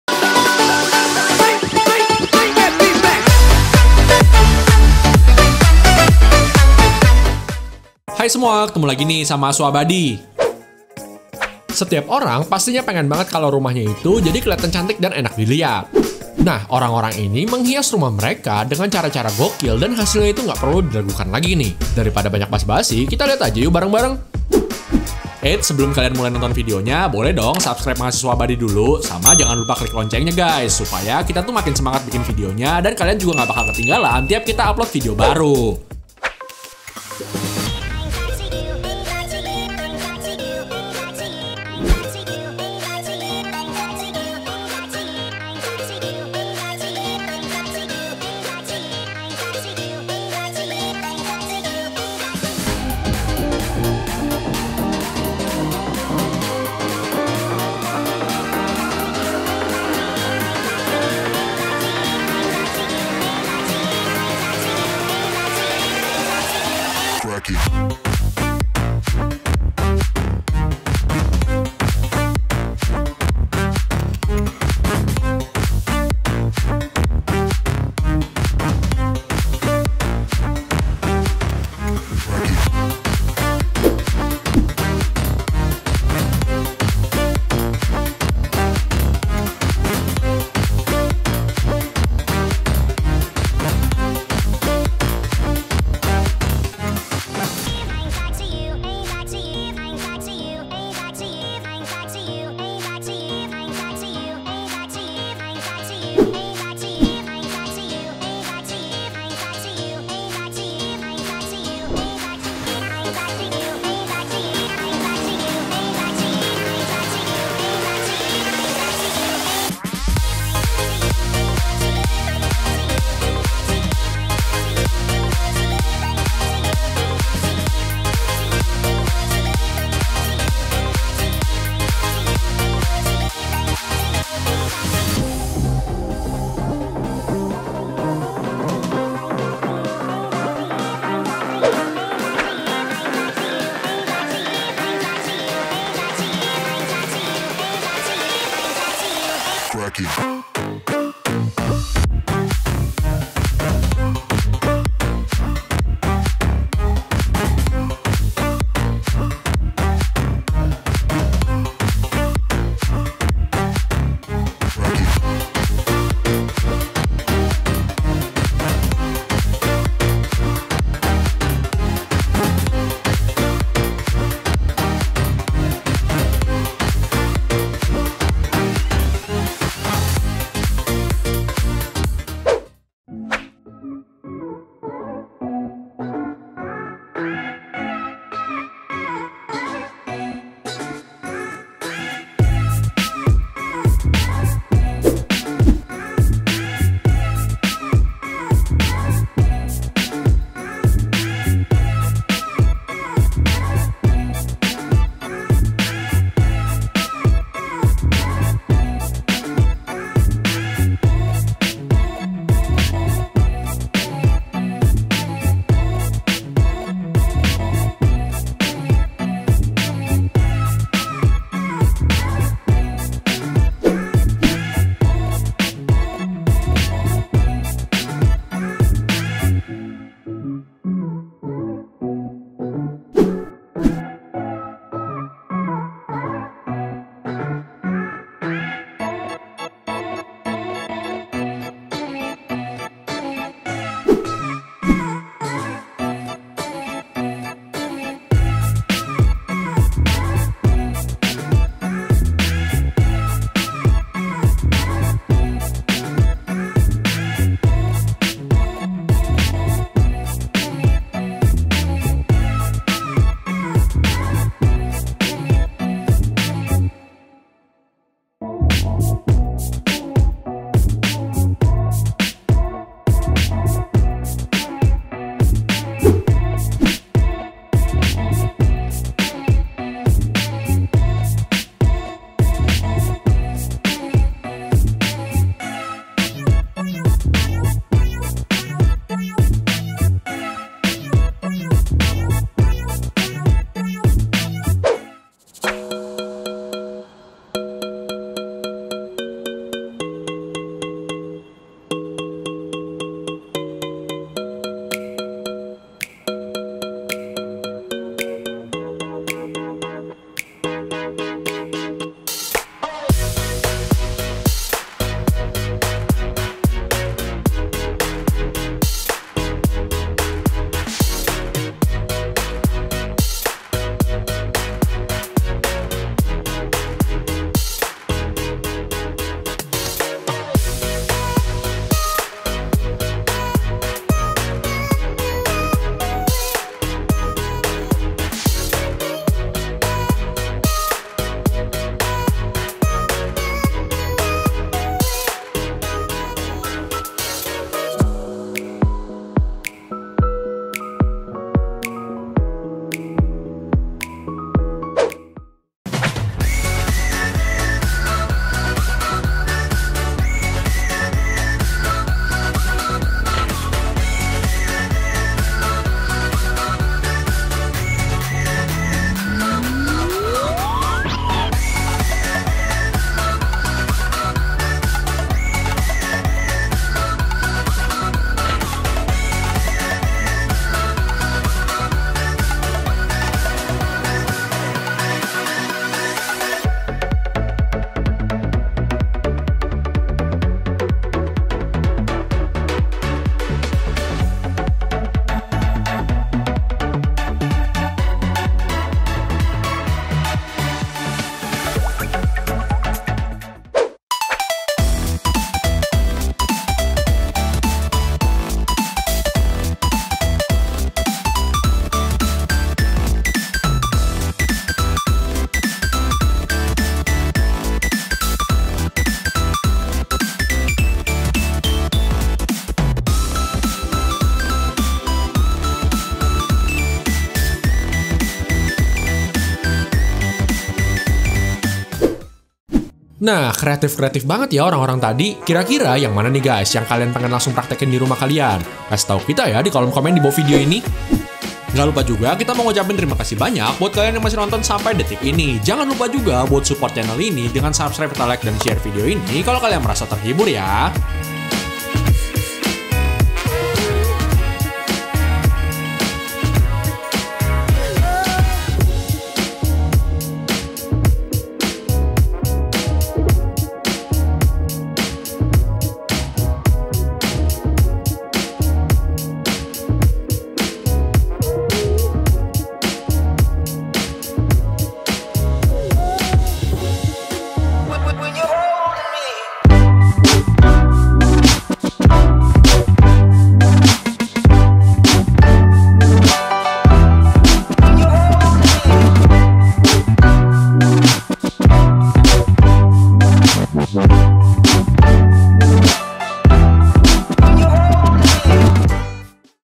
Bukan Sulap Bukan Sihir, Dalam 5 Detik Aja Rumah Bisa Disulap Jadi Rumah Baru Pake Alat Ini
Terkadang kita suka bingung cari ide gimana cara bikin rumah kita jadi keren kaya orang-orang kan ? Kali ini kita mau kasih lihat kalian Beberapa cara gokil yang dilakukan orang pada rumahnya yang hasilnya itu keren banget. Bahkan dalam 5 detik aja rumah ini disulap jadi rumah baru guys.